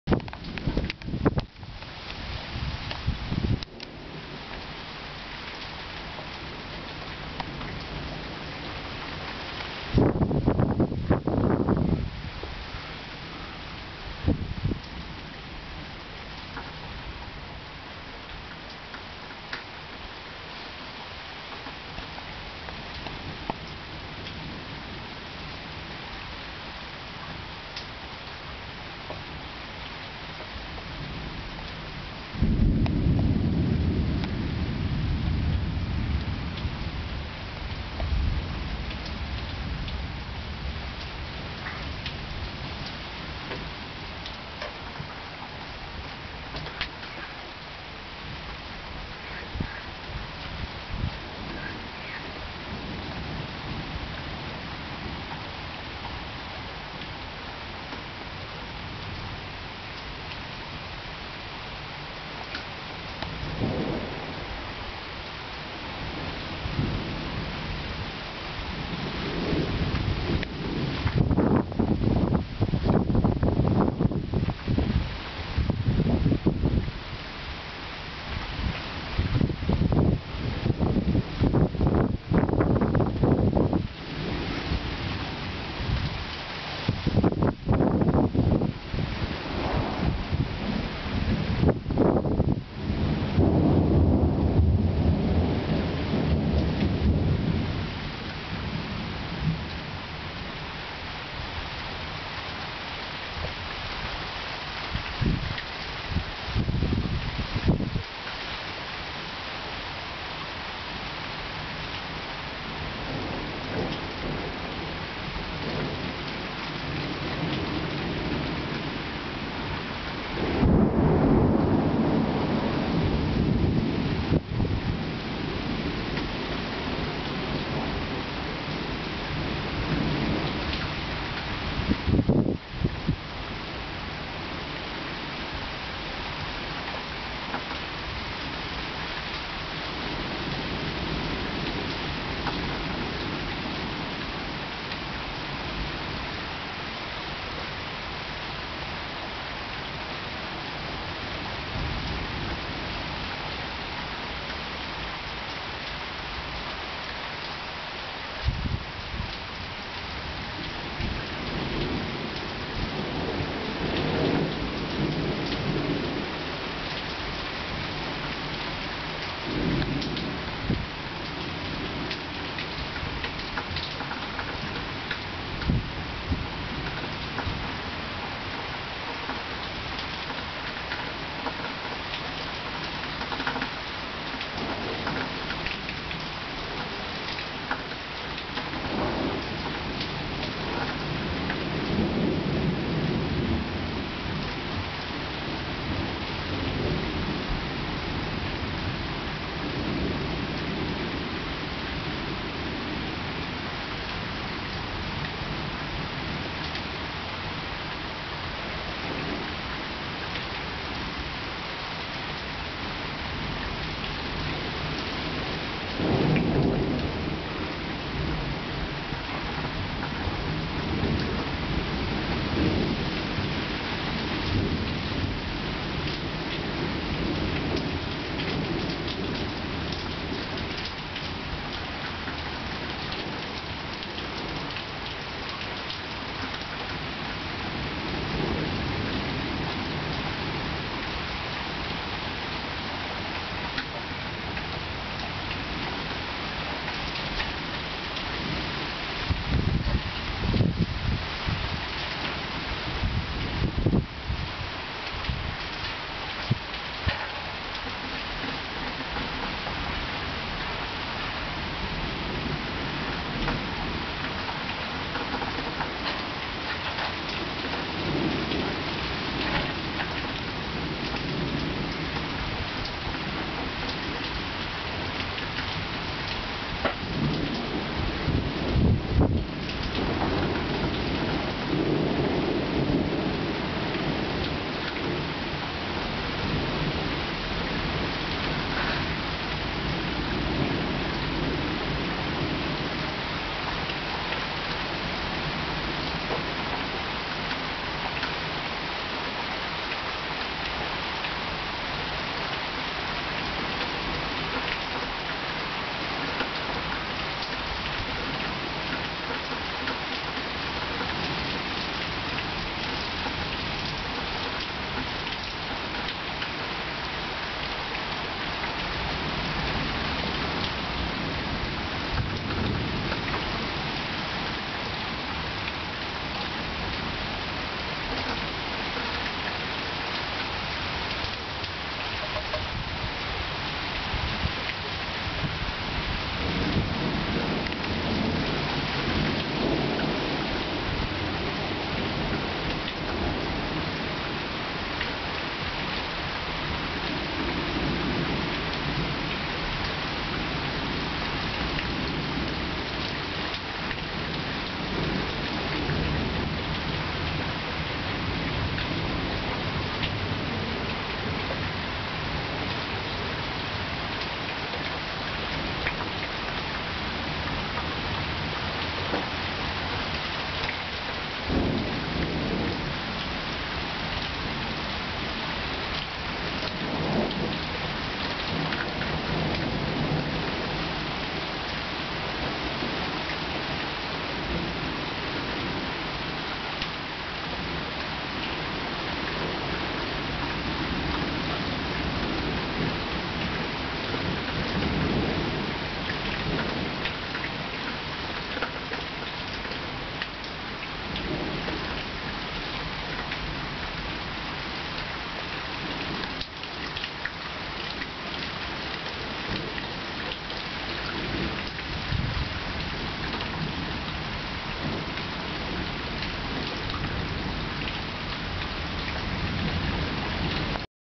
Dachbodenfund. Altes Video von einem Sommergewitter über Deutschland
Ich habe noch das alte Video in den hintersten Ordnern meiner Festplatte gefunden.
Ein Sommergewitter über Deutschland.
Aufgenommen im Juli 2010.

Ein kurzes, aber schönes Video vom erleuchtetem Himmel mit  untermauertem natürlichen Donnergrollen, welches begleitet wird von hypnotisch sanftem, fast lotrecht fallendem Regengeprassel.